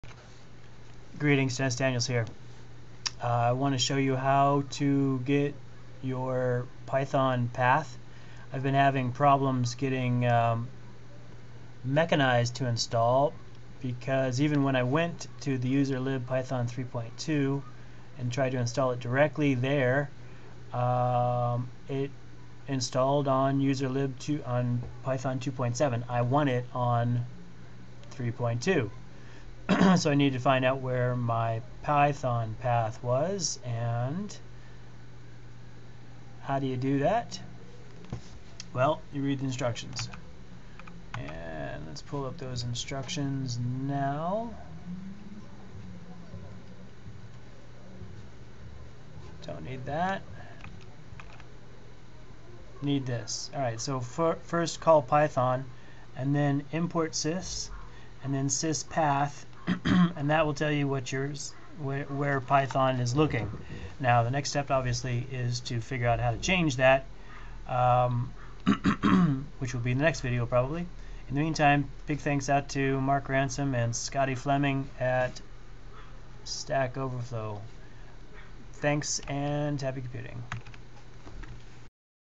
finding your python path on Linux Mint 13 Maya (64bit)
I wanted to do this

but discovered that easy_install was putting stuff into my 2.7 python and not 3.2 python...
so I had to verify my path ergo this video.

next:
Obviously, how to override my python path with what I want: python3.2!!